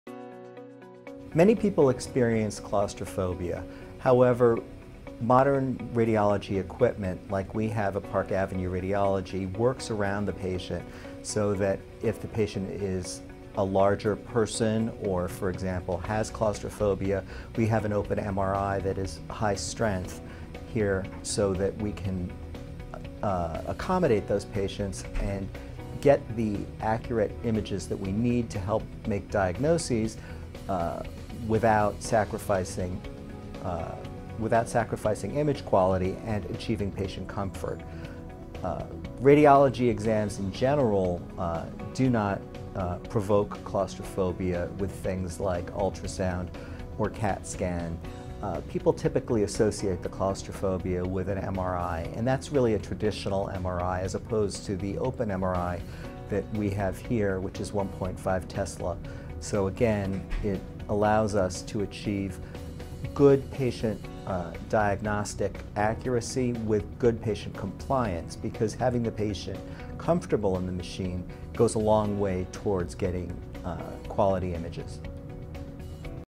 Does Claustrophobia Affect a Radiology Procedure? Radiology NYC
Dr. Marc Liebeskind, Medical Director at Park Avenue Radiologists, PC in New York answers common questions about claustrophobia and radiology procedures.